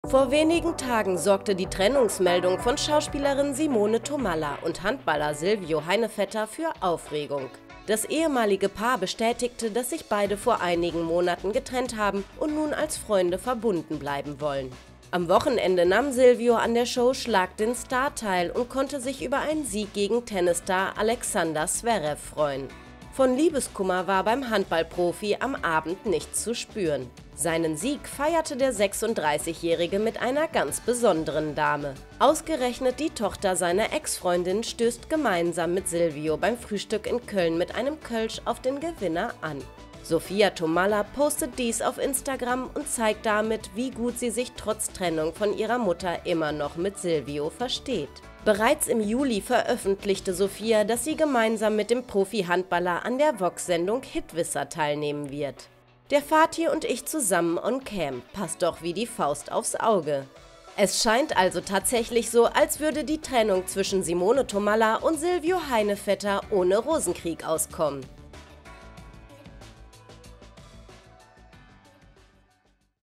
Simone Thomalla: Mit dieser Frau feiert ihr Ex Silvio nach Trennung
Gerade erst wurde bekannt, dass die langjährige Beziehung zwischen Simone Thomalla und Silvio Heinevetter gescheitert ist. Nun zeigt sich der 36-Jährige beim Feiern mit einer ganz besonderen Dame.